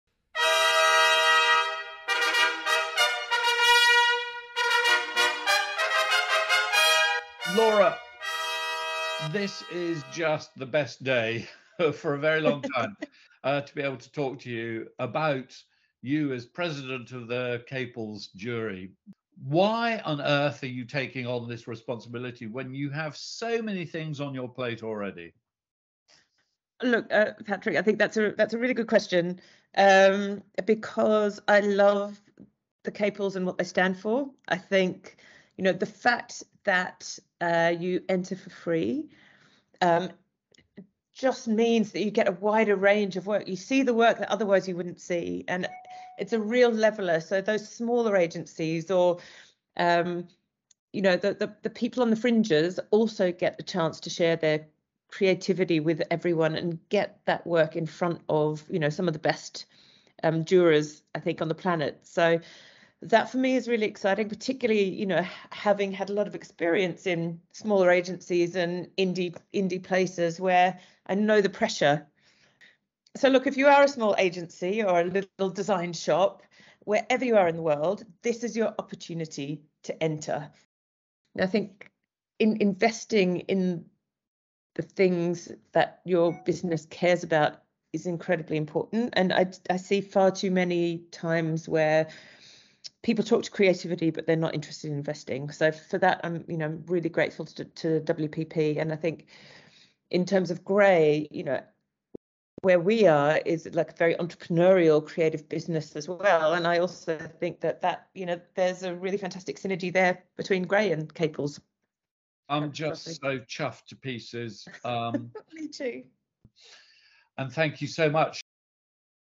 Laura JB - President of The Caples Awards 2023
Laura JB loves The Caples because it levels the playing field for small agencies who don't have the big awards budgets of network agencies. ENTER THGE CAPLES is her message!